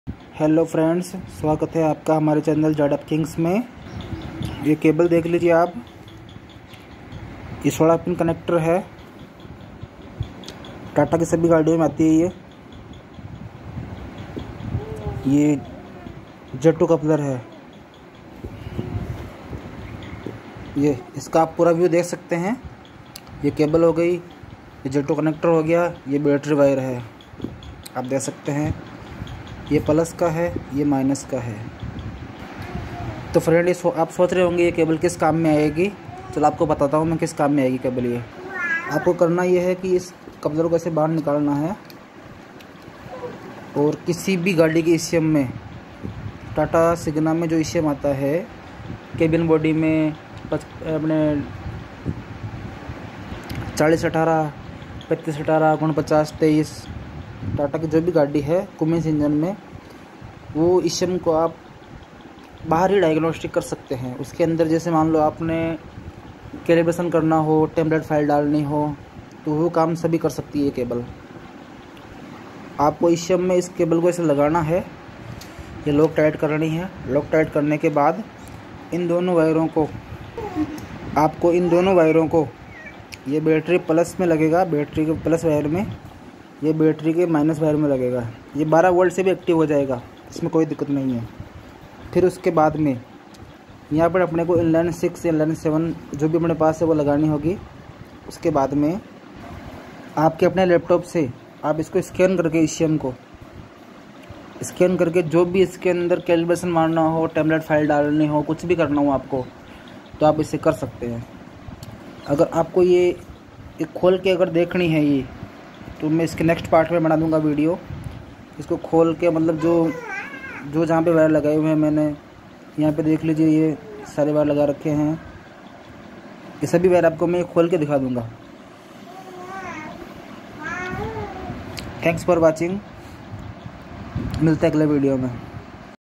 How too unlock ecm without vehicle ! / ecm ko vehicle ke bina scan kaise kare
aaj hum aapko Tata cummins vehicle ke ecm ko bina vehicle ke direct scan karne ka tarika sikhyenge too aap is video ko pura deke aur sikhiye

agar aaspko iski wiring ka diagram chahiye too
mujhe  comment kare me aapko iske agle.part
part 2 me isko khol ke bata dunga

Indian trucks
# Tata vehicle
# Tata bs6
zf kings
Tata commercial vehicle